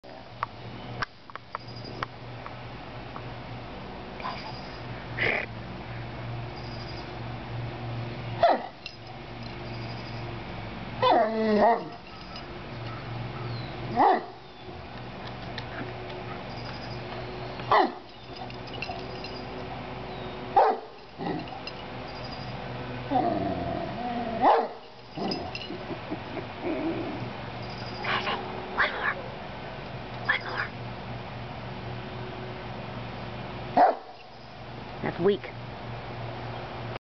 Daisy the talking Irish Wolfhound
Big Daisy, the Irish Wolfhound, who is now in heaven, loved life and loved to talk.  We all miss her!!  She was 185 pounds of fun and love!!